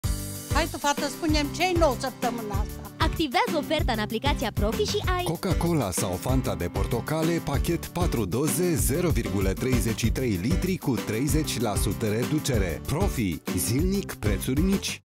Activează oferta în aplicația Profi și ai: Pachet Coca Cola sau Fanta de Portocale cu 30% reducere.
Activează oferta în aplicația Profi și ai: Coca Cola sau Fanta de Portocale, pachet 4 doze, 0.33l cu 30% reducere. Oferta supusa unor termene si conditii. Profi,zilnic preturi mici.